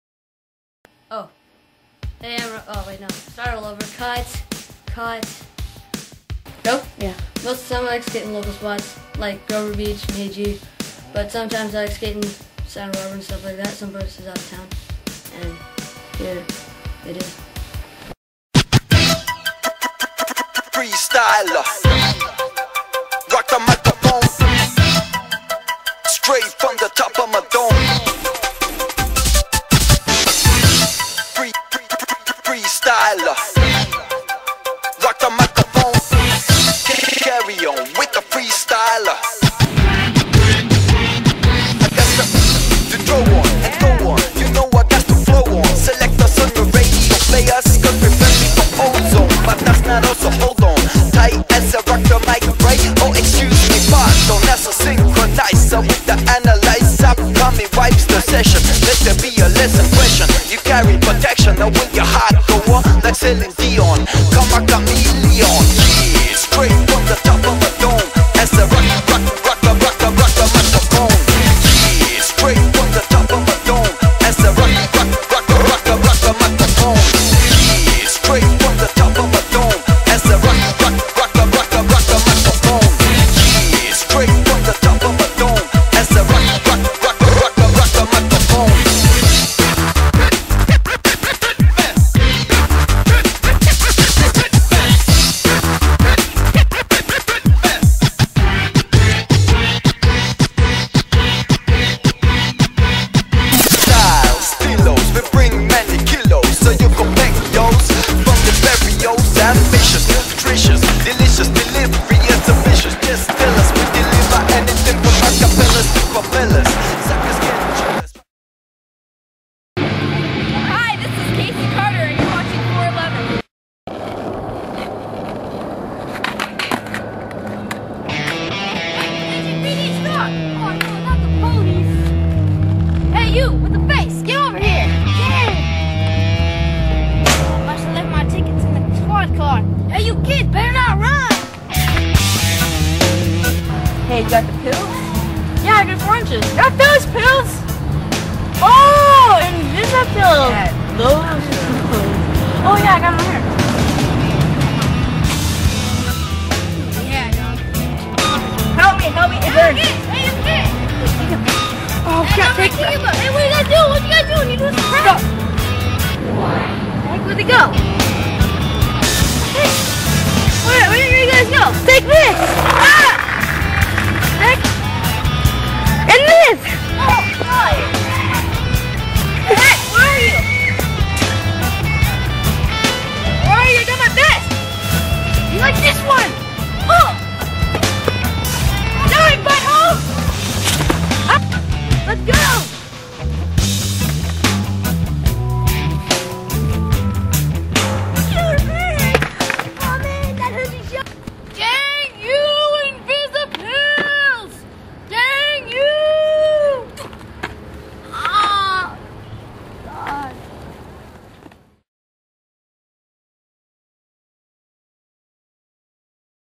Part 2 Pablo's and Invisi-Pill
Ryan "Pablo" Preciado's skate part and Invisi-Pill Comericial.